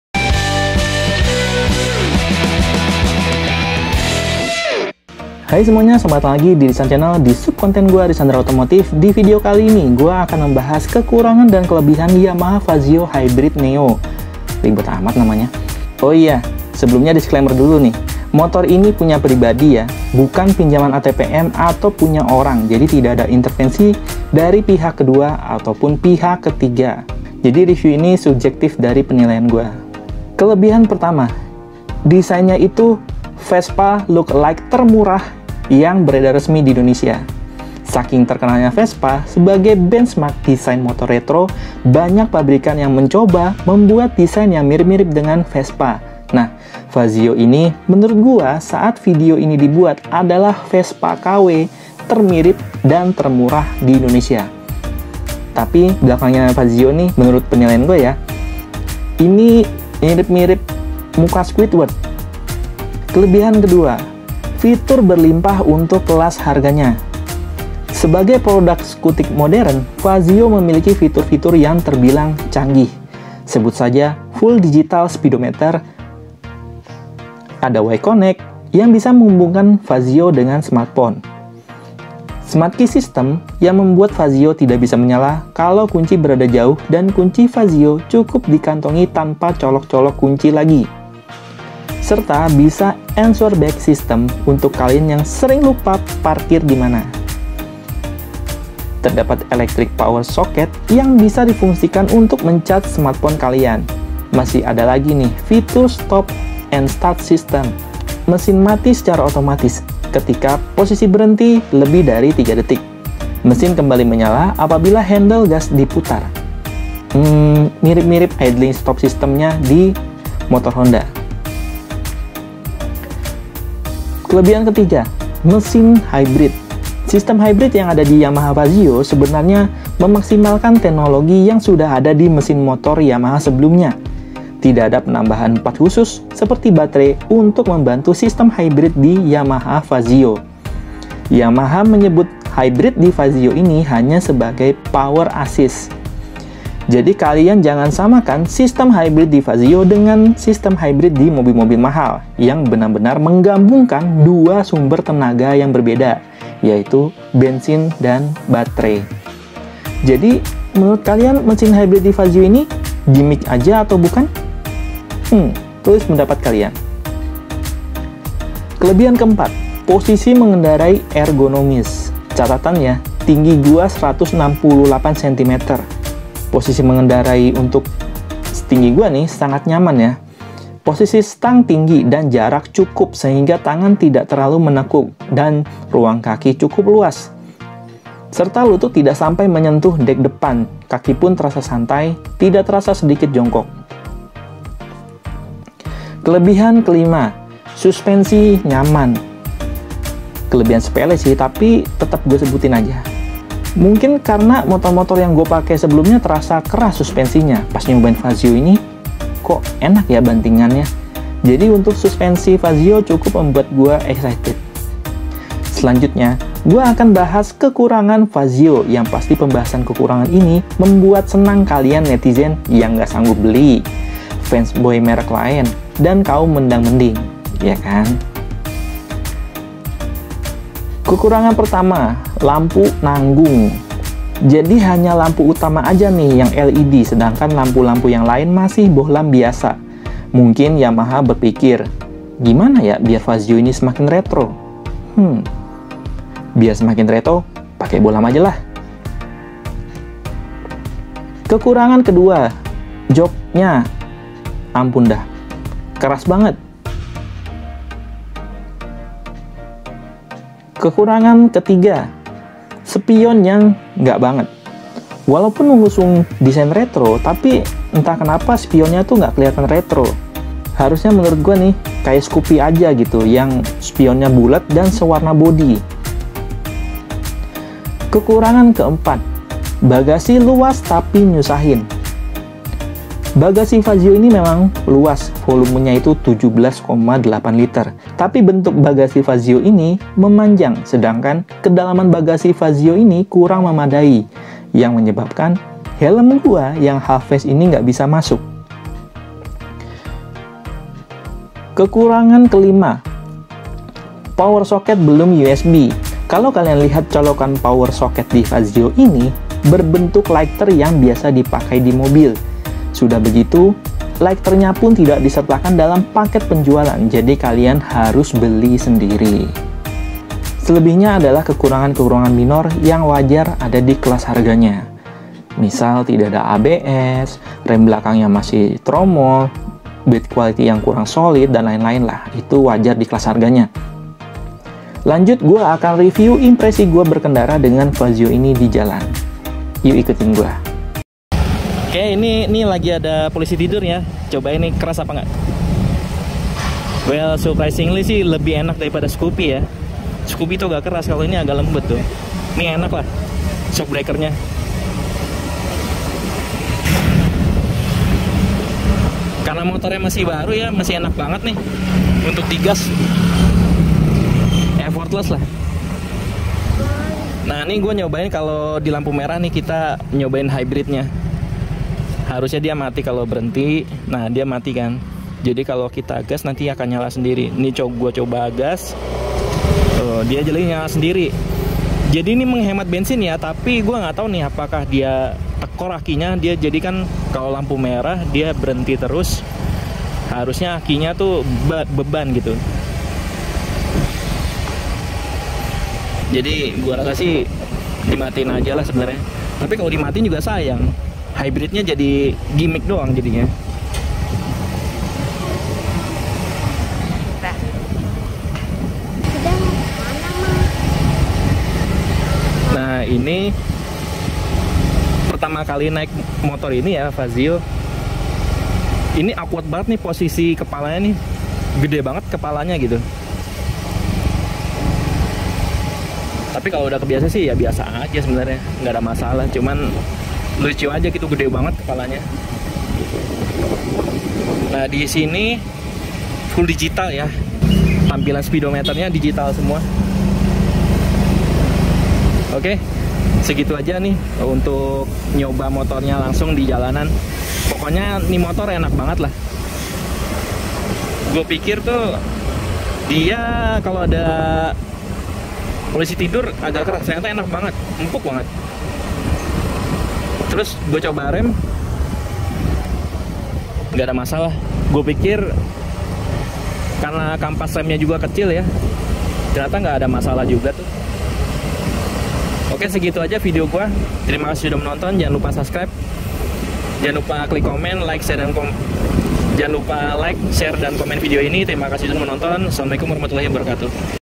Yamaha Fazzio Hybrid Neo - kok begini ya?
Yamaha Fazzio adalah salah satu skutik Yamaha yang mengusung desain berkonsep klasik, Harga Yamaha Fazzio Hybrid Neo berkisar 22 juta OTR. Dengan dilengkapi banyak fitur modern, Seperti panel instrumen full digital, keyless system, Y-Connect, serta teknologi hybrid yang tersemat di mesin Yamaha Fazzio. Apakah Yamaha Fazzio ini bisa memuaskan gw? simak video gw berikut ini.

Risandra Channel dibuat untuk memberikan informasi seputar gadget, otomotif, kuliner, jalan-jalan, dan horror dengan sederhana dan jujur apa adanya tanpa gimmick-gimmick yang aneh-aneh.